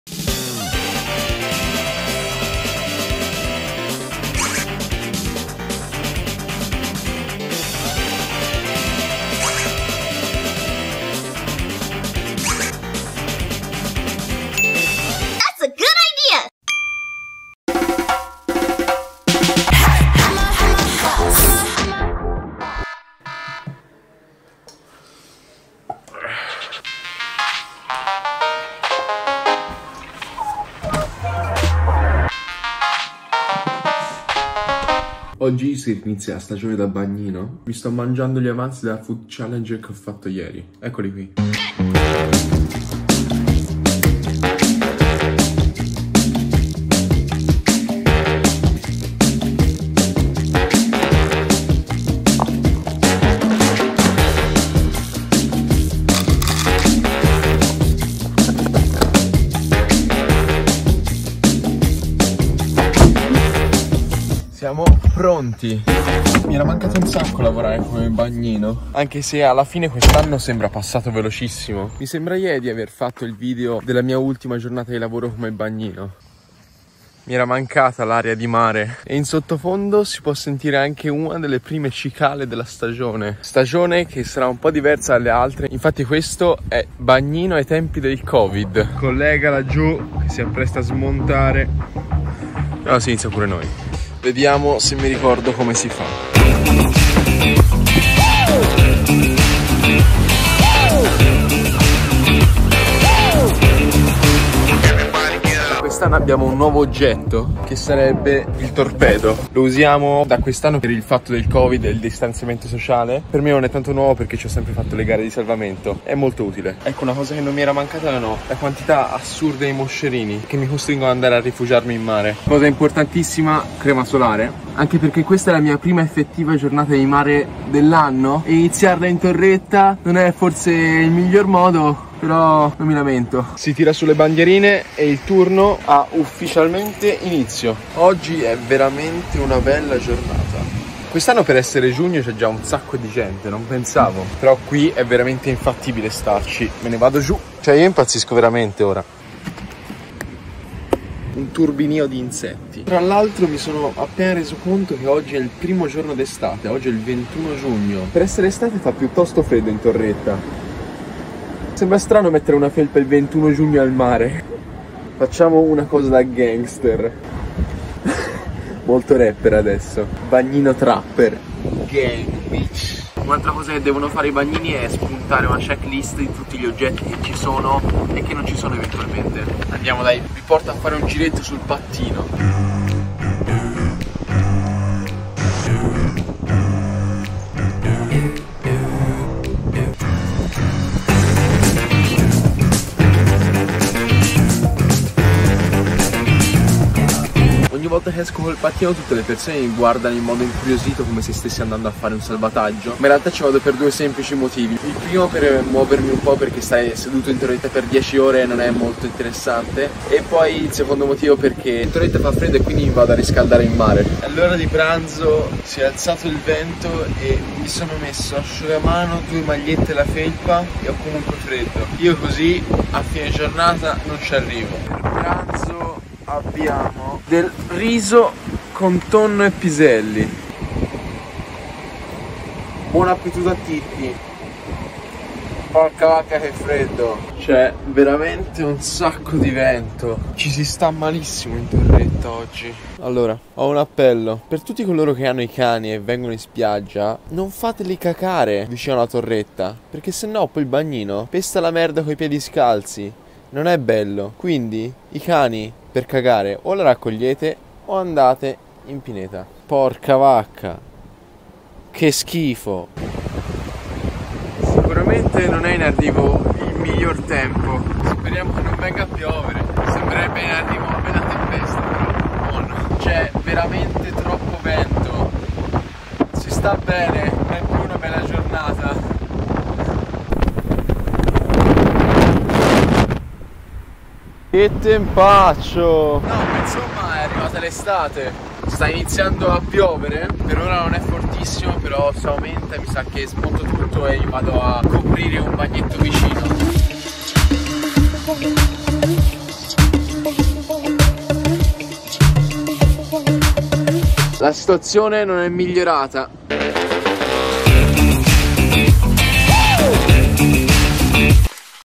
PRIMA GIORNATA DA BAGNINO - estate 2020 | lifeguard DITL #09
UN GRANDE RITORNO! anche quest'anno si torna a sorvegliare le spiagge! questa è la mia prima fantastica giornata di lavoro come bagnino per quest'estate,le spiagge sono dominate dalle mascherine e il distanziamento sociale? assssolutamente NO! Tutto normale e BBBellissimo as always bb.

BUONA VISIONE!